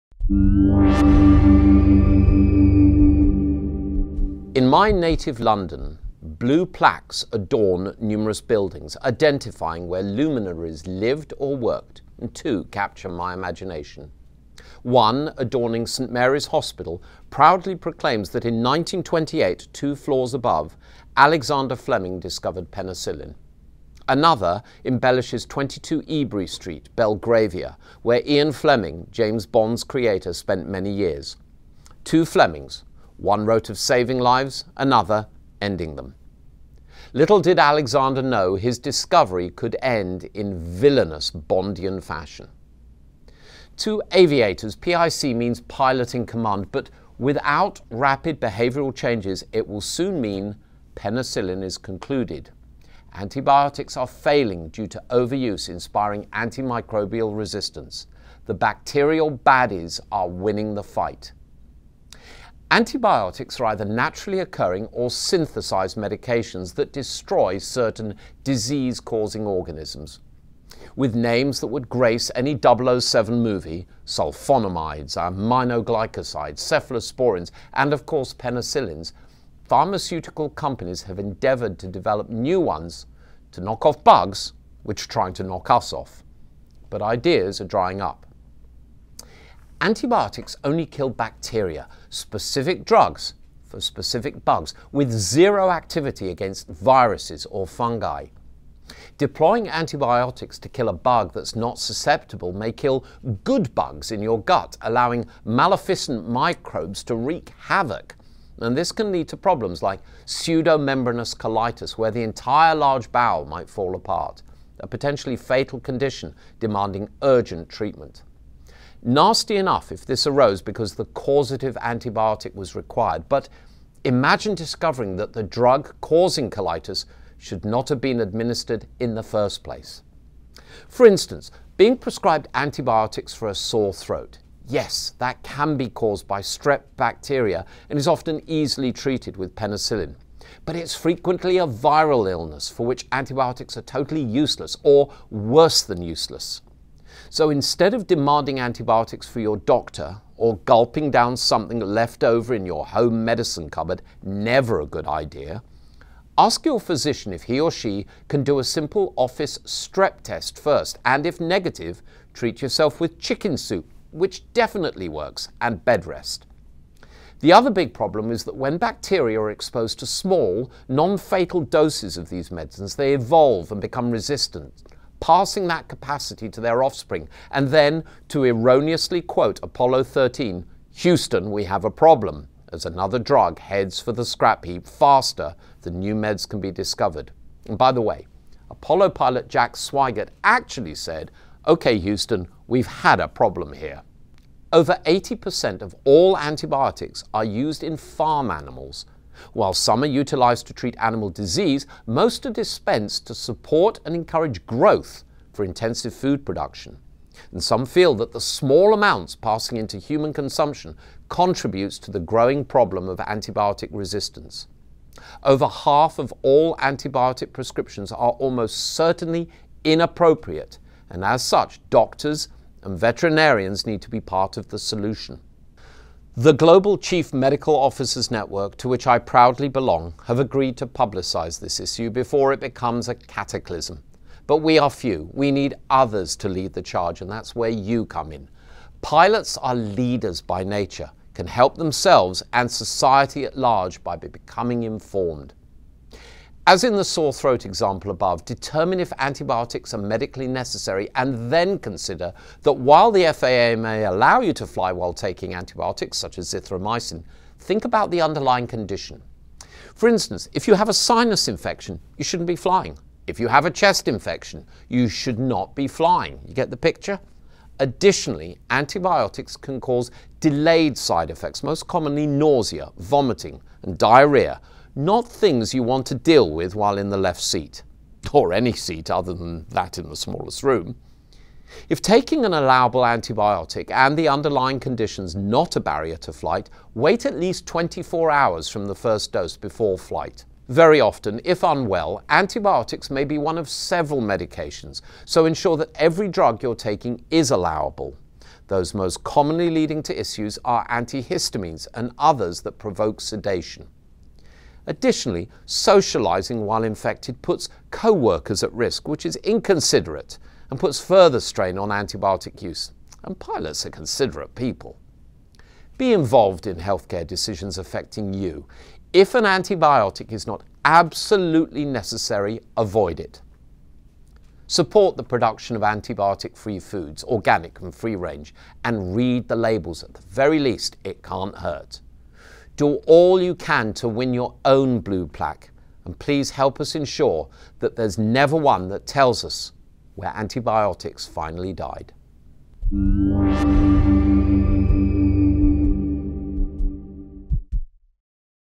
Fly Well 4 - A tale of two Flemings- Antibiotics
Maybe you shouldn't fly while taking an antibiotic